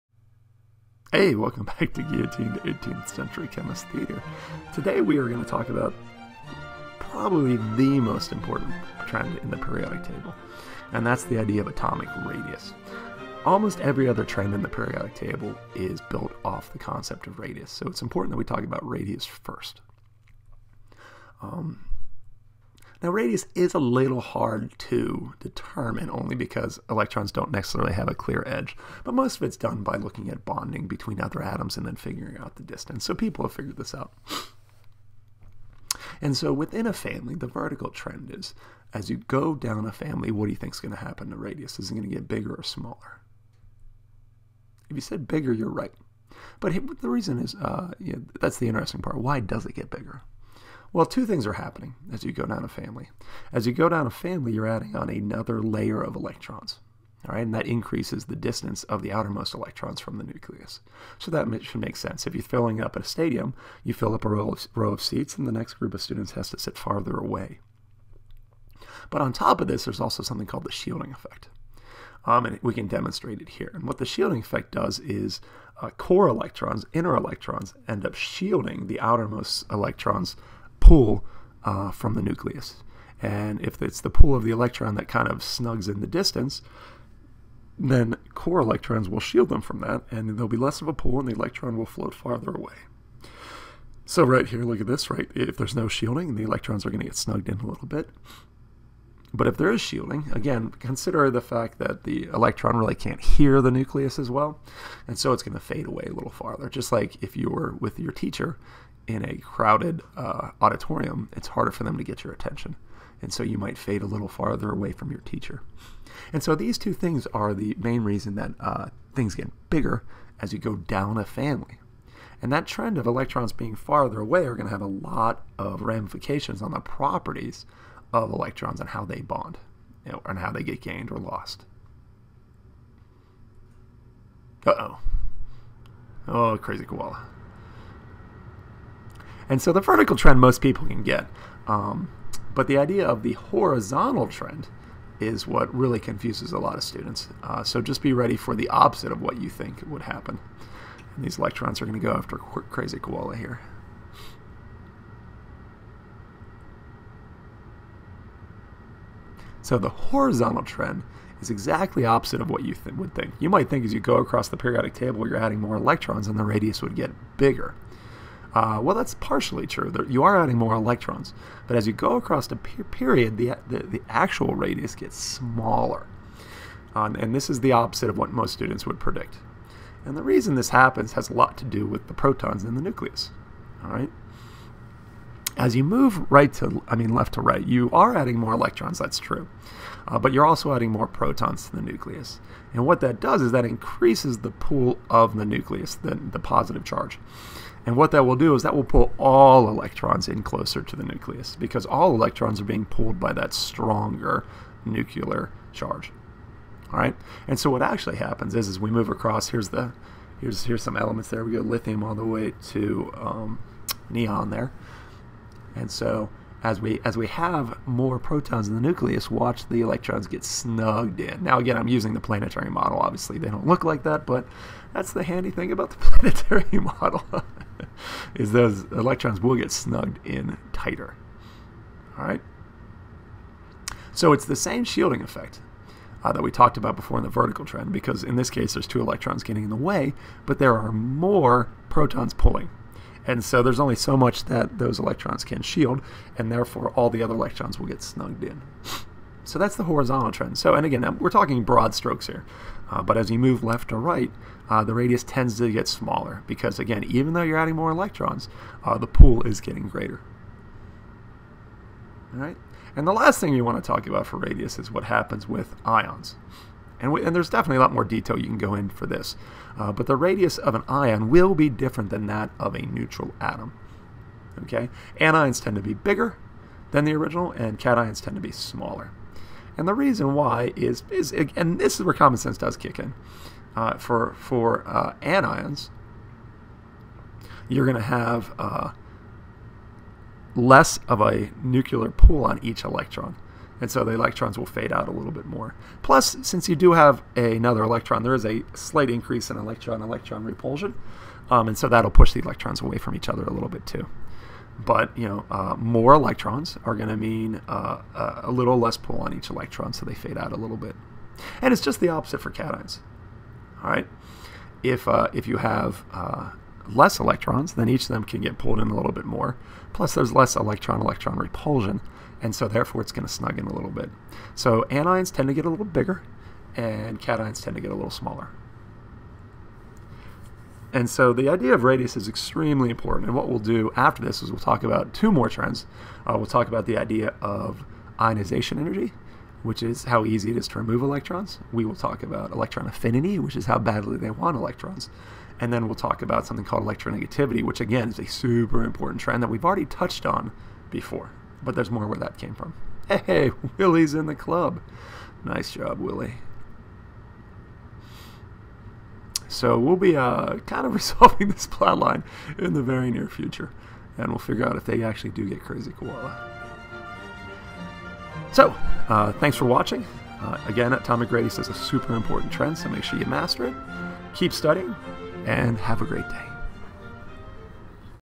Modern Atomic Theory: Atomic Radius Trends
All hail RADIUS! If you want to understand why an atom does what it does, take a look at those outermost electrons (a.k.a. the valence electrons). You can look at how many there are, but you can also look at how far away from the nucleus they are. Let's show the periodic trends for that.